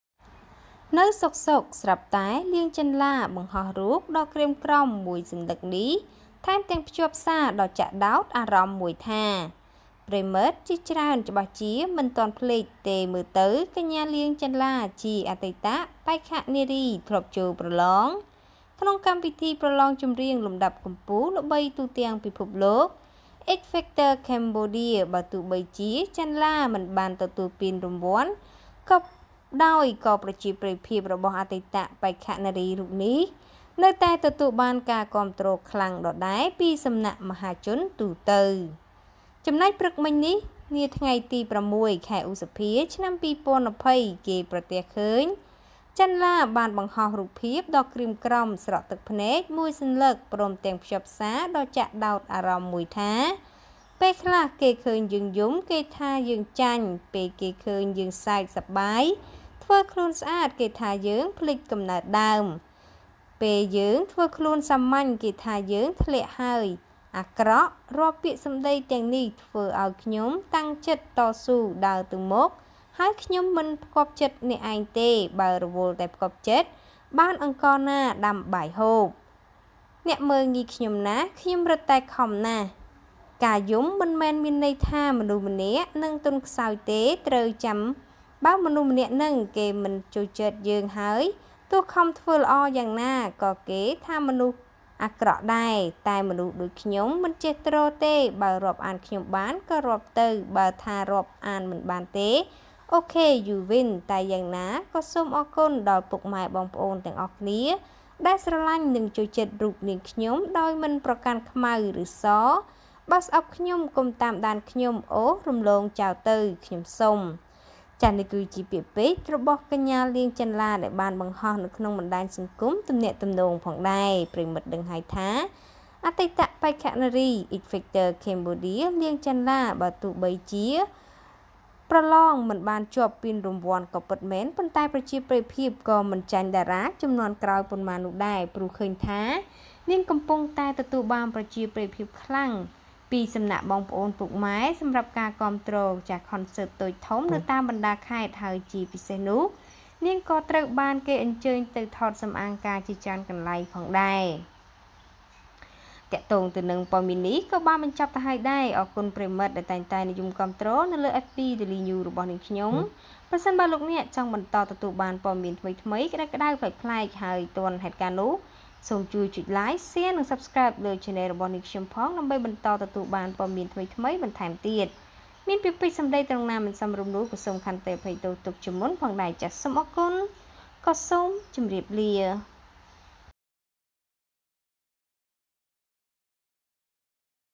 លៀង ចាន់ឡា បង្ហោះរូបដ៏ក្រៀមក្រំមួយសន្លឹកនេះ ថែមទាំងភ្ជាប់សារដ៏ចាក់ដោតអារម្មណ៍មួយថា
នៅសុខៗស្រាប់តែ លៀង ចាន់ឡា បង្ហោះរូបដ៏ក្រៀមក្រំមួយសន្លឹកនេះ ថែមទាំងភ្ជាប់សារដ៏ចាក់ដោតអារម្មណ៍មួយថា…លៀងចាន់ឡា